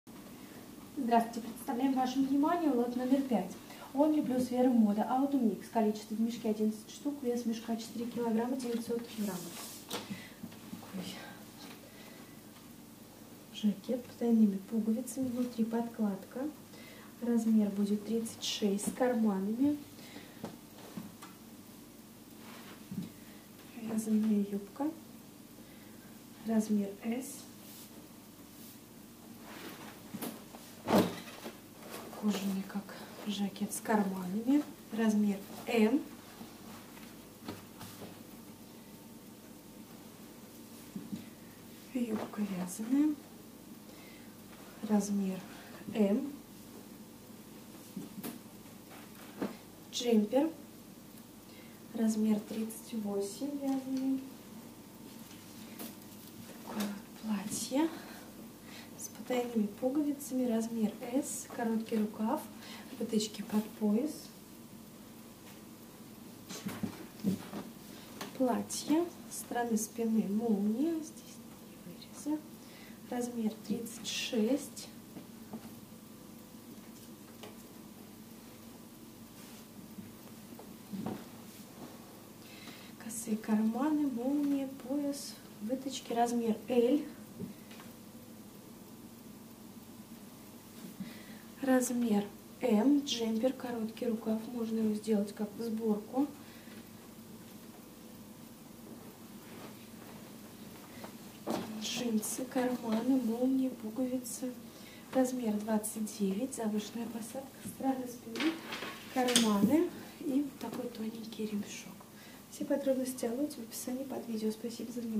Only + Vero Moda Autumn mix 5,Цена за кг: 20.09 евро, сток одежда оптом
Only + Vero Moda Autumn mix (лот№5)
Микс женский  (сток)
Страна: Европа
Демисезон
Цена за кг.: 20.09  евро
Цена за мешок: 98.44 евро
Вес мешка, кг.: 4.9